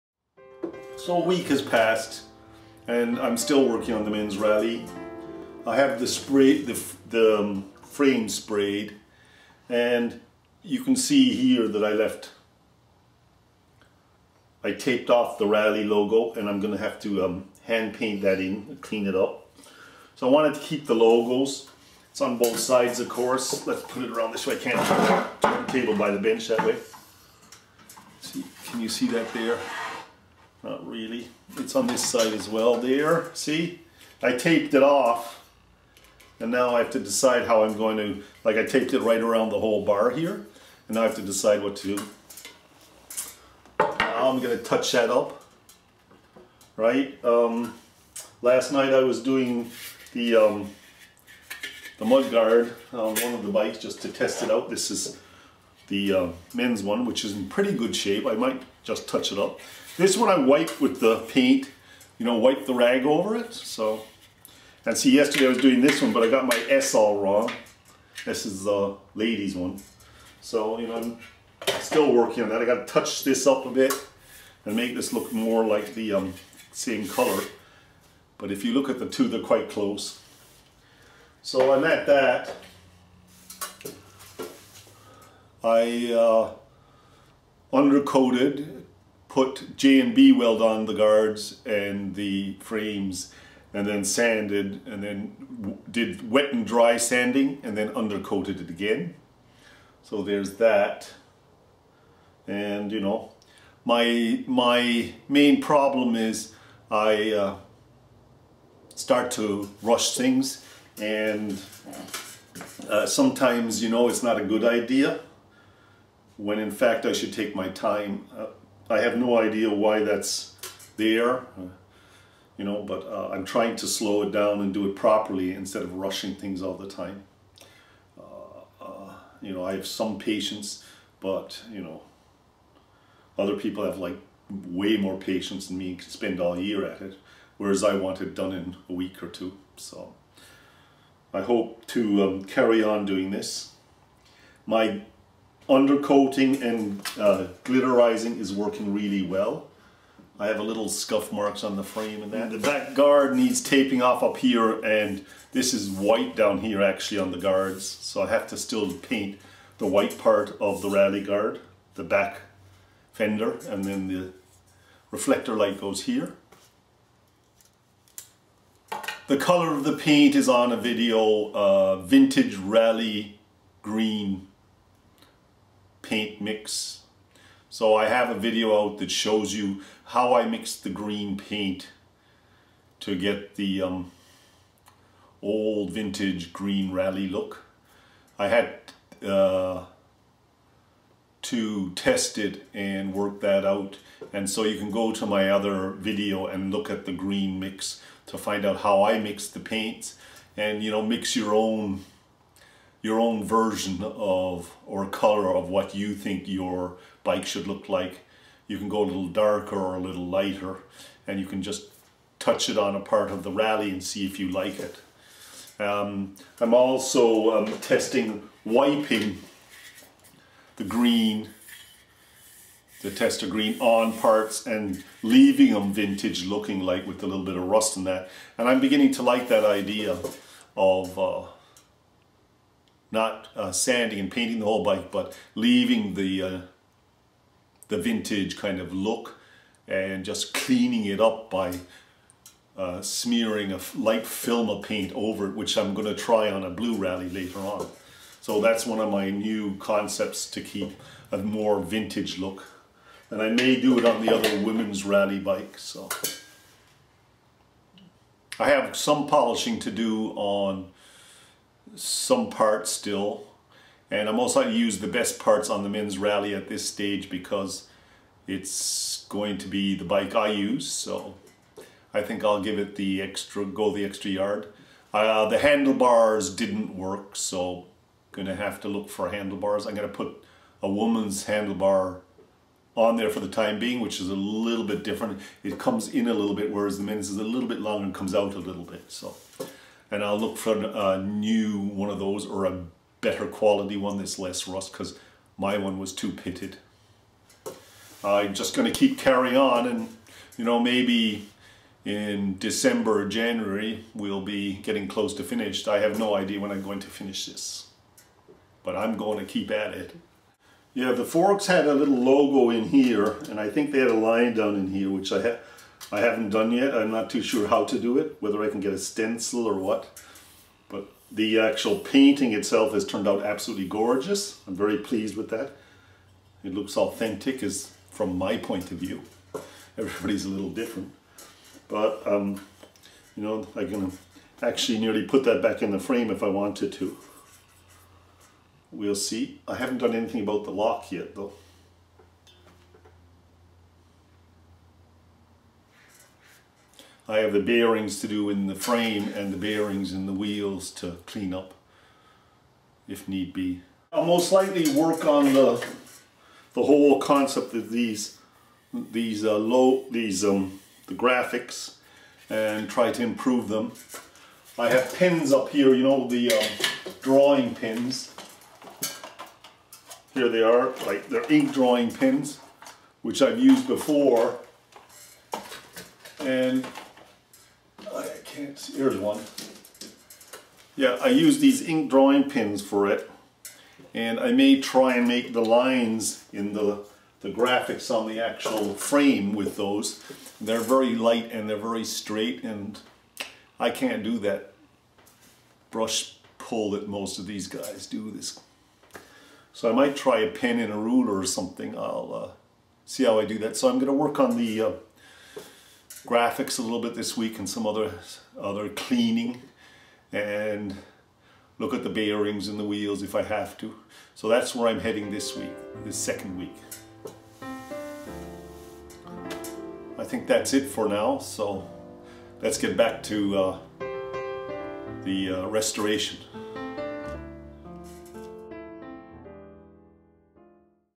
VINTAGE BICYCLE RESTORATION #4
After one week of working on the vintage Raleigh bicycle I have had to slow down and I have decided to do a really good finish to this bicycle and not rush it.